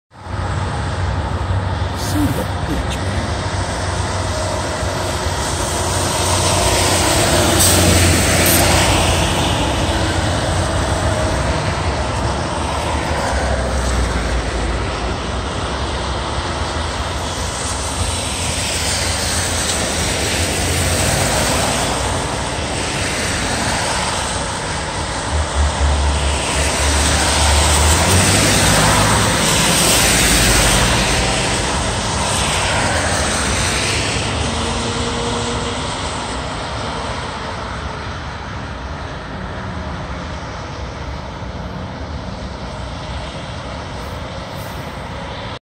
Nearly missed this CSX Intermodal in Woodlawn because Jacksonville doesn't use proper signaling
Glendale, Ohio Wednesday, February 14th, 2018 about 2:45 p.m. . The Tropicana switcher had just going south on track two and there was no indication that anything Northbound was coming. I left right away and was on Glendale Milford Road in Woodlawn when I heard a horn blast at Marion Road and could not believe that a Northbound was headed this way. My only choice was to pull over at Anthony Wayne Avenue at the light and try to film along the side of the road as the train passed over the Glendale Milford bridge.